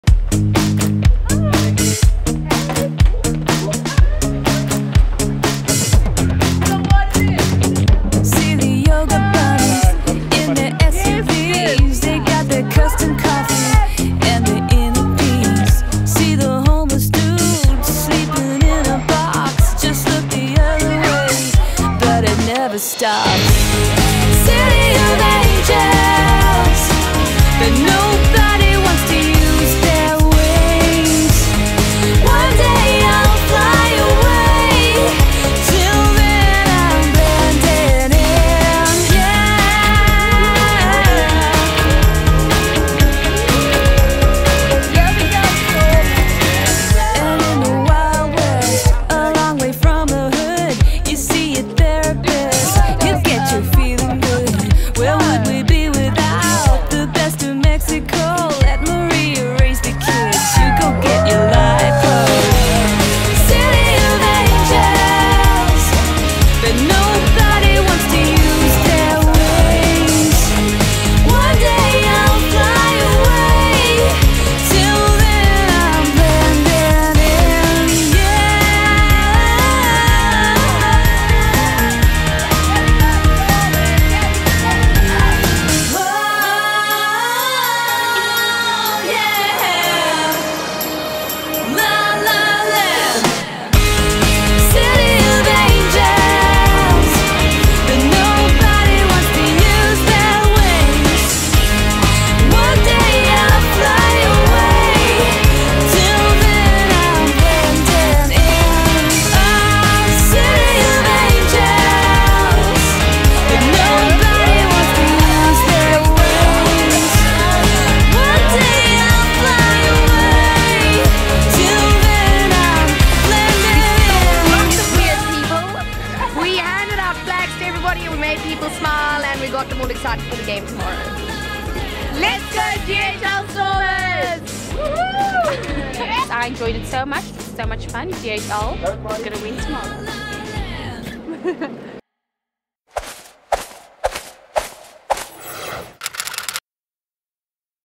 DHL Stormers Girls around Cape Town
The DHL Stormers cheerleaders danced around Cape Town handing out DHL Stormers flags.